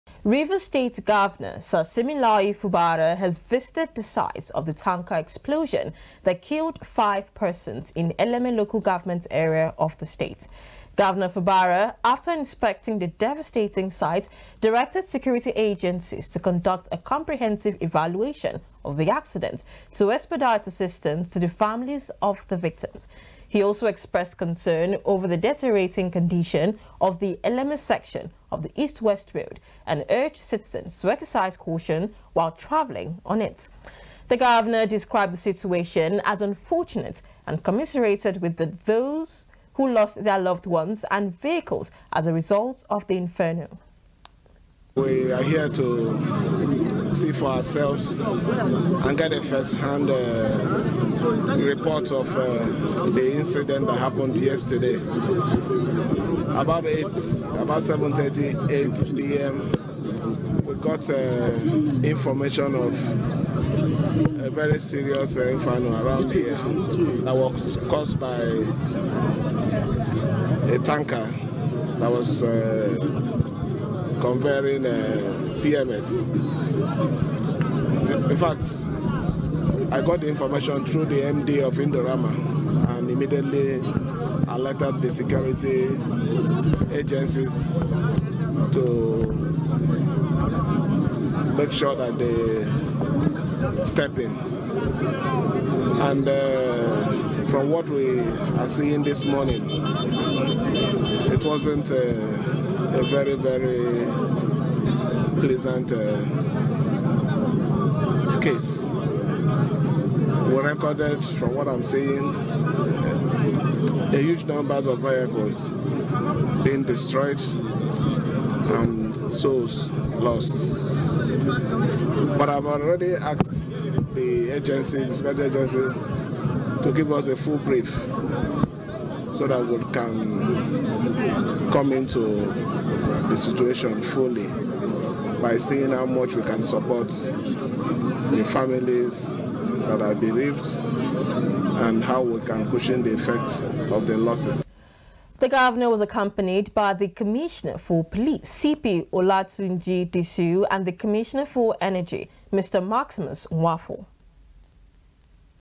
GOV FUBARA EXPRESSES GRIEF, TO SUPPORT BEREAVED FAMILIES OF EAST-WEST ROAD FIRE DISASTER
Rivers State Governor, Sir Siminalayi Fubara has expressed grief over the fire disaster on the East-West road that caused loss of lives and destroyed several vehicles on Friday night.

ROLL VIDEO/ Governor Fubara after inspection of the carnage that occured at Aleto, Eleme area near Indorama Petrochemicals Limited, noted that when he got information about the fire incident on Friday night he quickly dispatched the security agencies to step in, describing the situation as unfortunate and commiserated with those that lost their loved ones and vehicles as a result of the inferno.